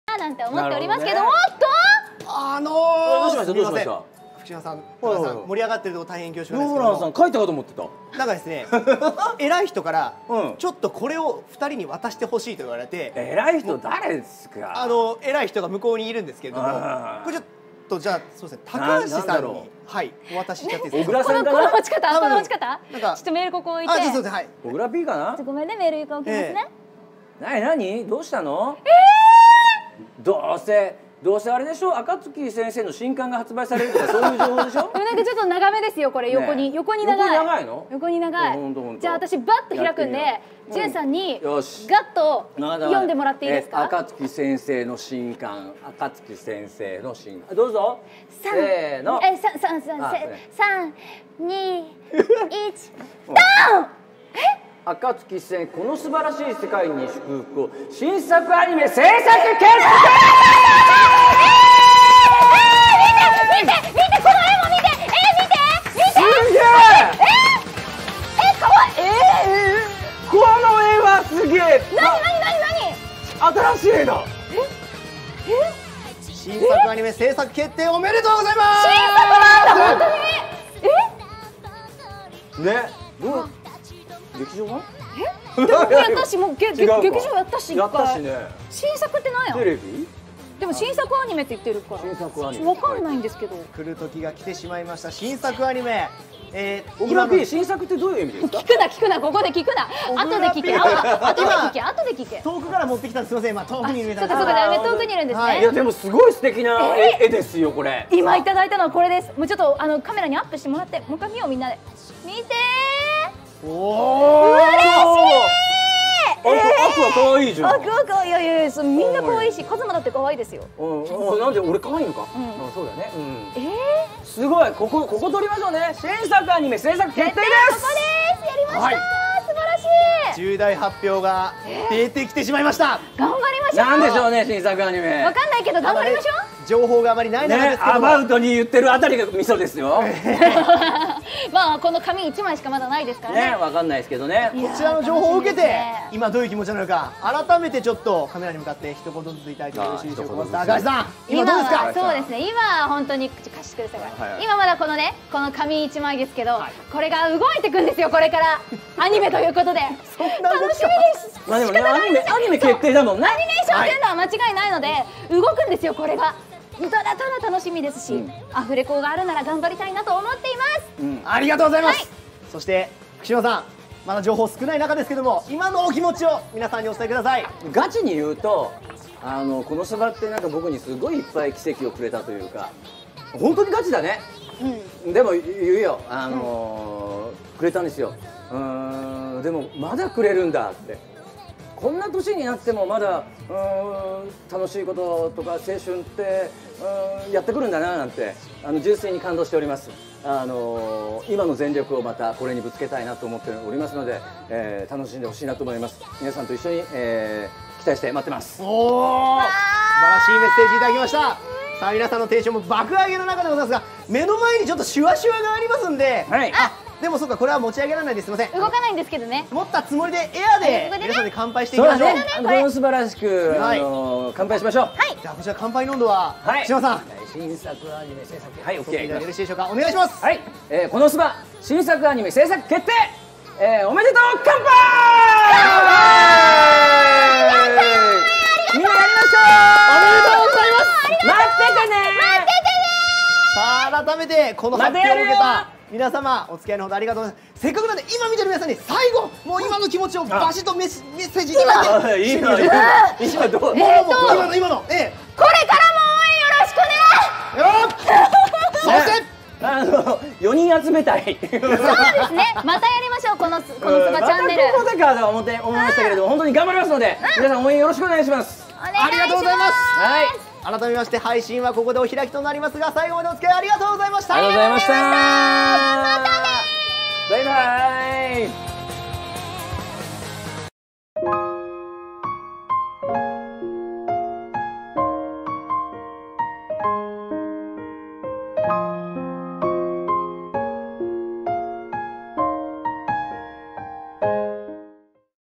KonoSuba Voice Actors React to New KonoSuba Anime Announcement (Which Turned out to be Season 3)
Fukushima Jun (Kazuma) and Takahashi Rie (Megumin) are presented some big news at the end of a KonoSuba live stream. A new anime production for KonoSuba has been announced!

(Please note that this video was from July 18, 2021)

To clarify, this just means there will be a new KonoSuba animated work of some sort. This does not necessarily mean it's going to be a 3rd season. It could be a movie, a new season, an OVA, or something else. We do not have any other info besides the announcement of production. But thankfully, it's not a pachinko!

Fansubbed by me, LEGOF. If there are any translation errors, feel free to point them out!